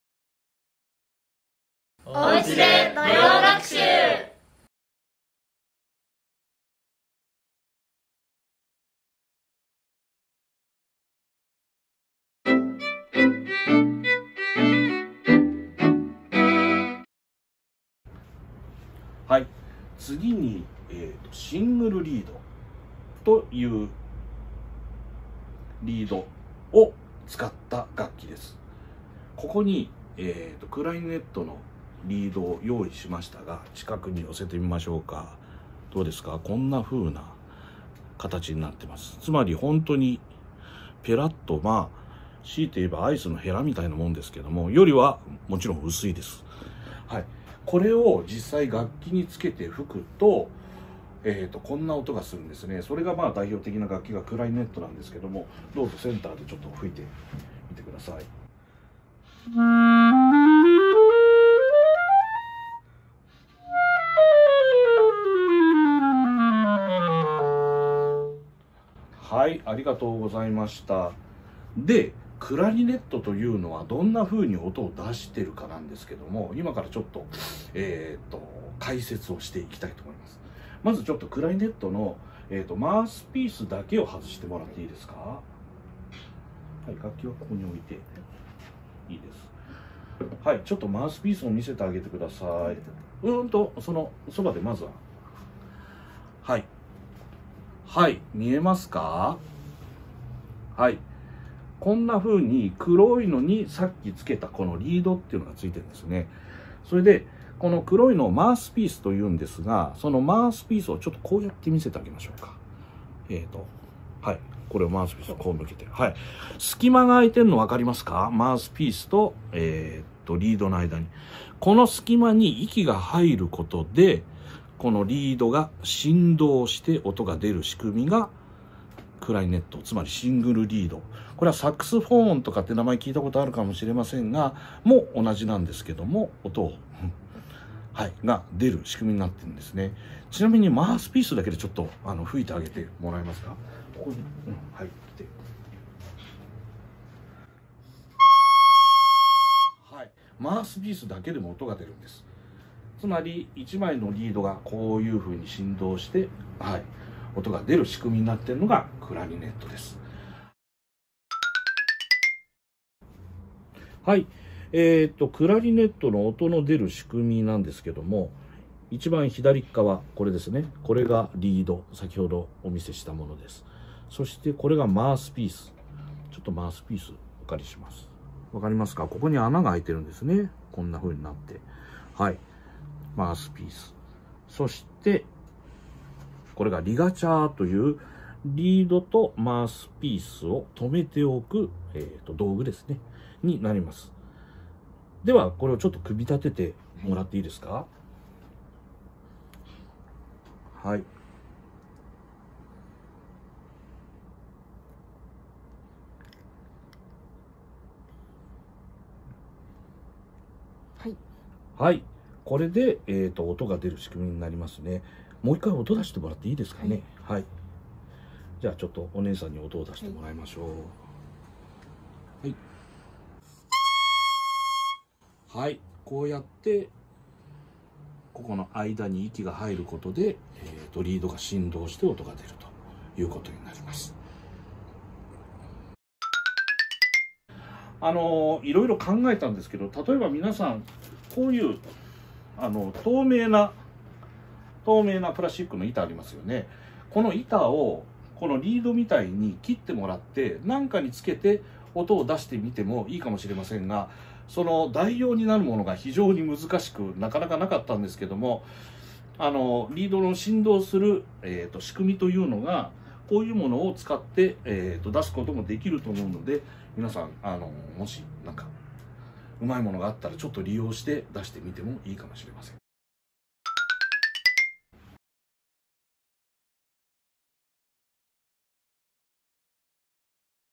楽器の音の出る仕組み作り（木管楽器② シングルリード）
名古屋芸術大学　依田嘉明先生
木管楽器の音の出る仕組み（シングルリード）について学びます。